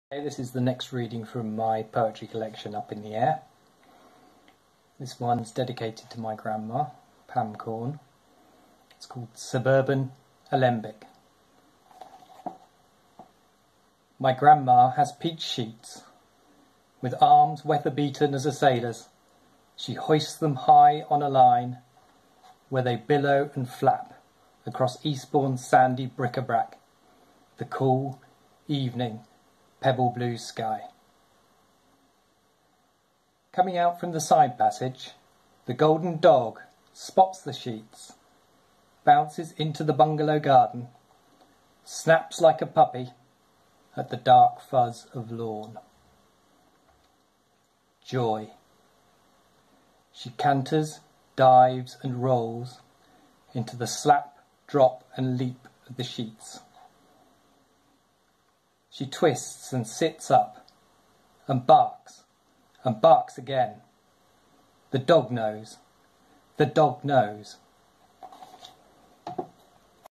Up in the Air - Poems on Video: Suburban Alembic
Here's the latest in my series of Poems on Video from my collection Up in the Air.

An alembic is a distilling tool that combines different elements to create something new. It's used in philosophy for a process of refinement, or transmutation. This poem, written about a summer evening in my grandmother's garden in Eastbourne, is my first video request. Let me know if there's a poem from Up in the Air that you'd like me to read on video.

And of course this poem is dedicated to my grandma, Pamela Isobel Lilian Korn.